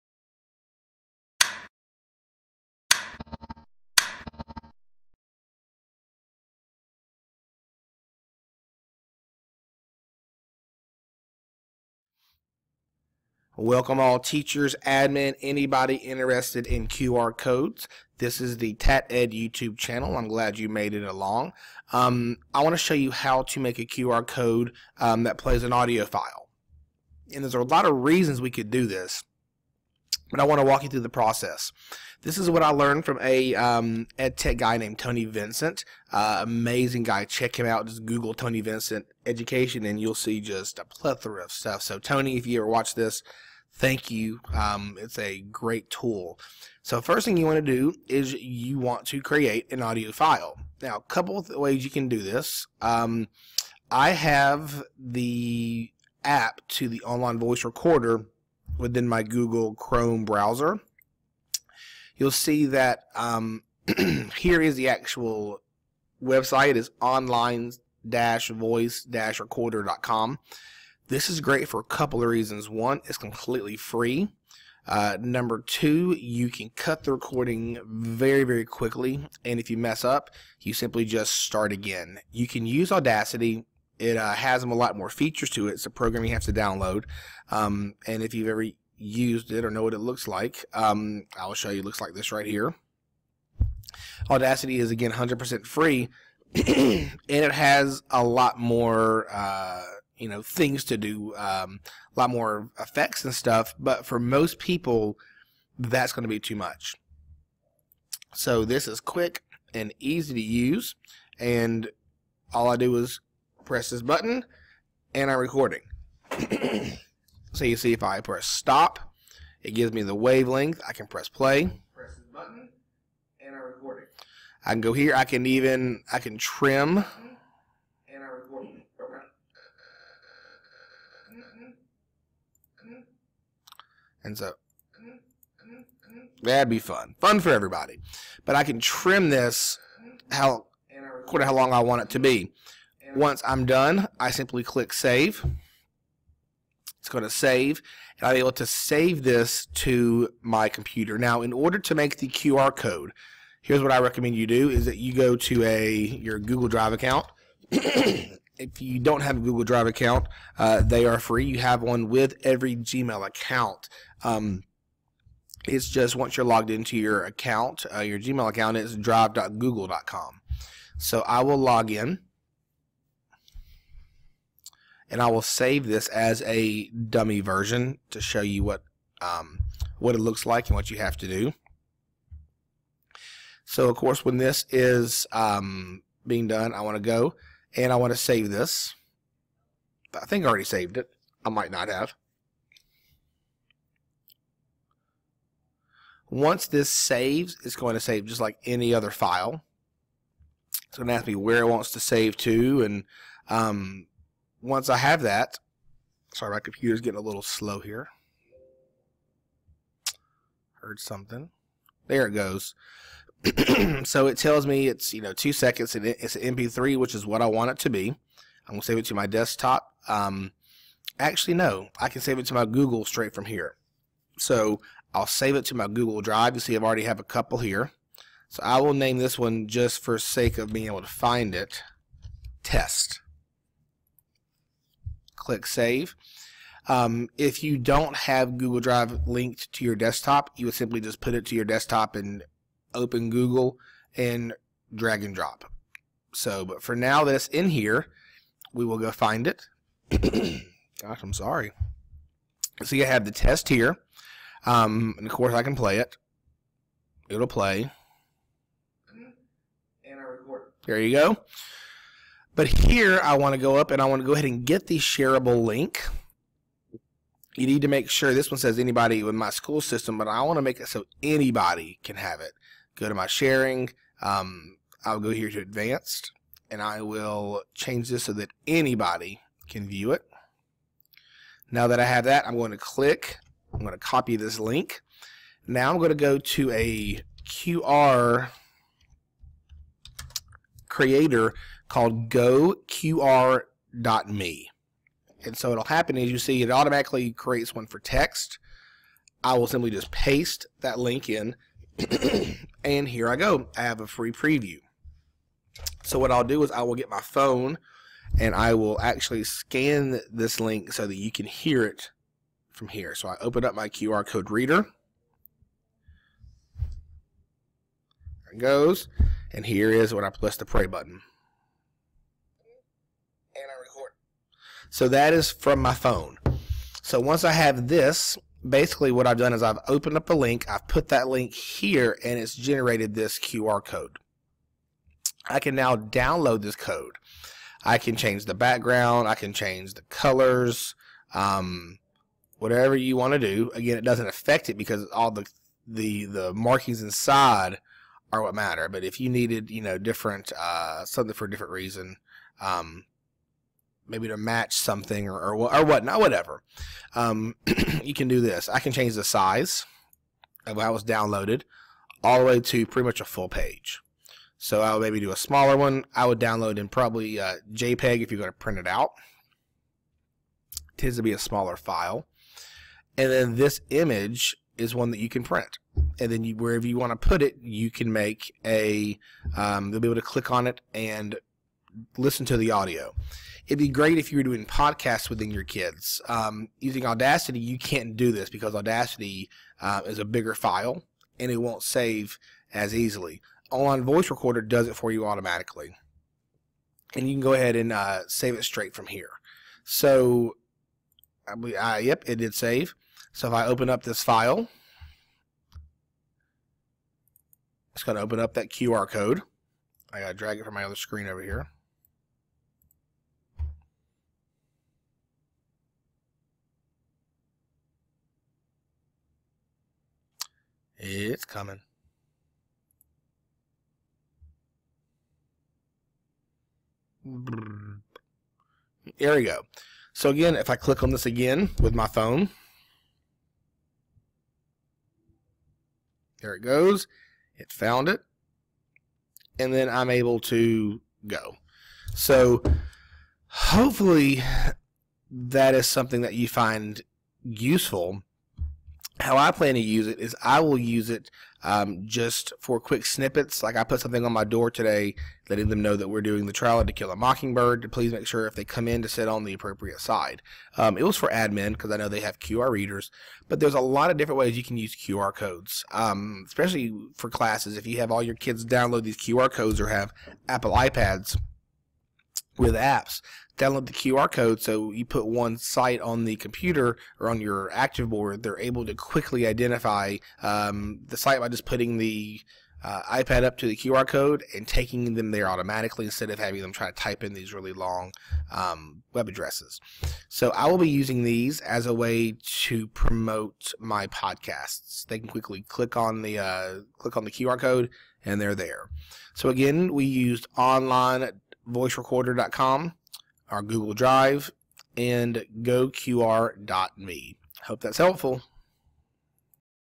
Create QR Codes that link to MP3's
This video explains how to create QR codes that will play mp3's. I learned about this from Tony Vincent